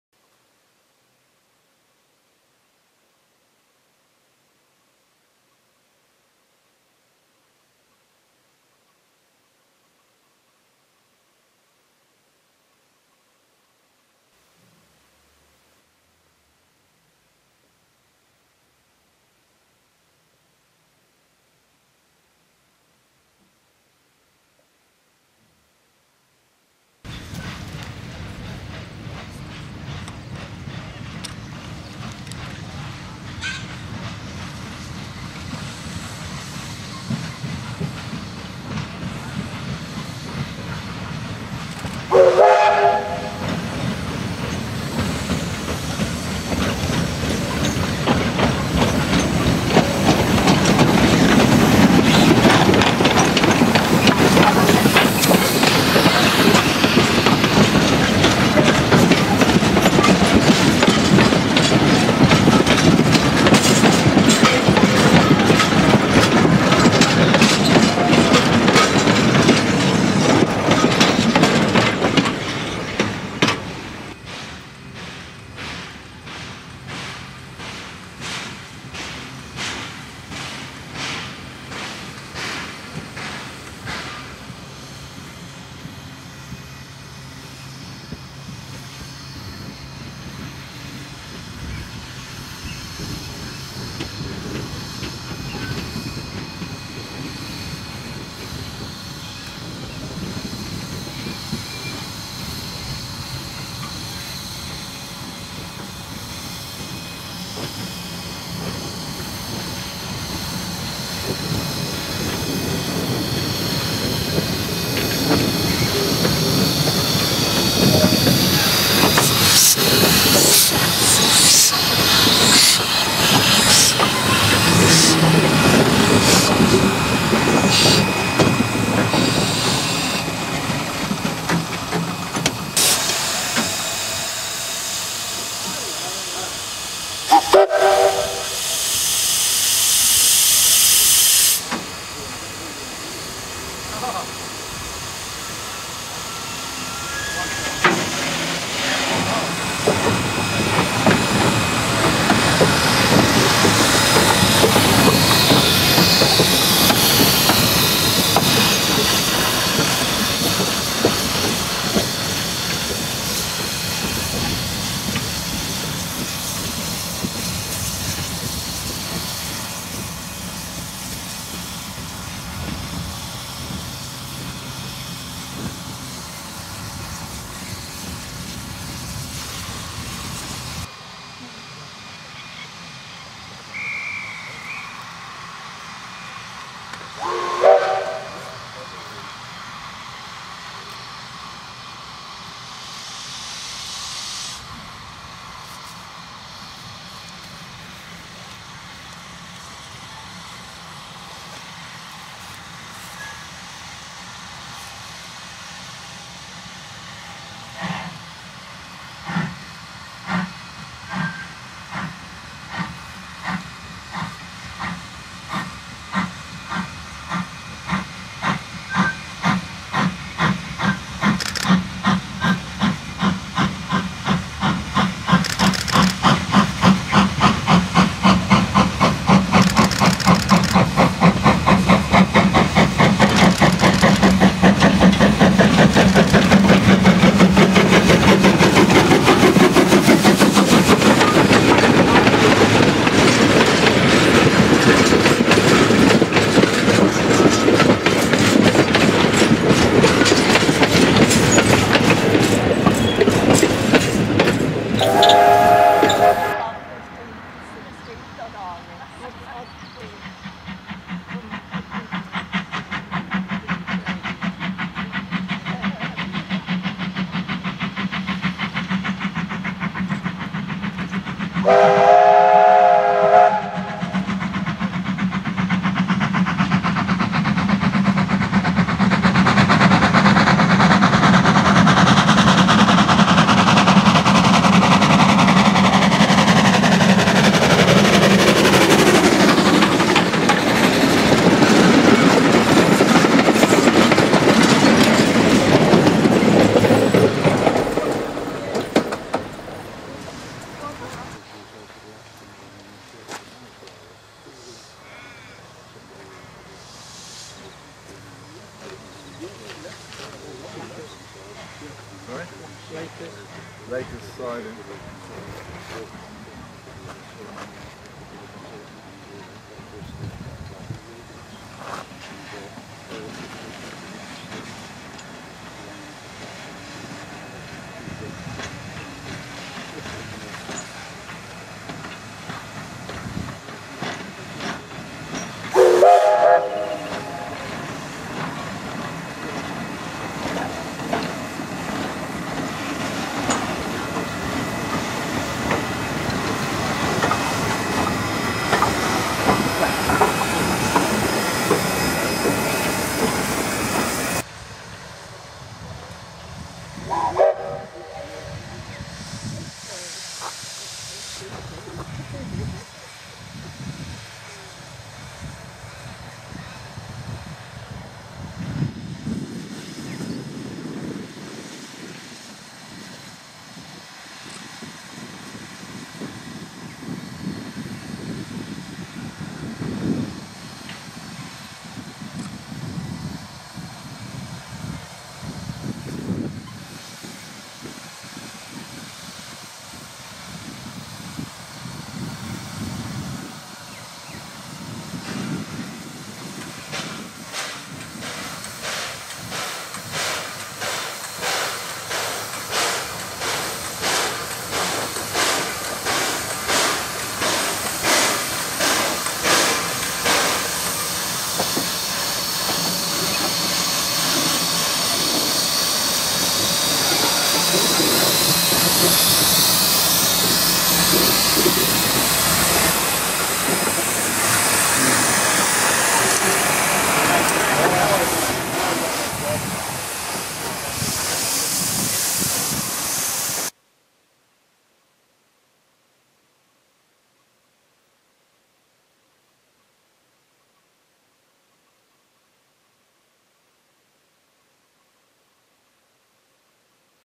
Bellarine Peninsula Railway
A short video showing operations on the 16km, 3' 6'' gauge line between Queenscliff and Drysdale, Victoria, Australia.
Locomotive T251, a 4-8-0 is seen arriving at Queenscliff and then running around it's train. A false start is made from Queenscliff for the benefit of photographers before a run-past is made further down the line. Finally, T251 is seen running around its train at Laker's Siding.
Filmed on 11th April 2023.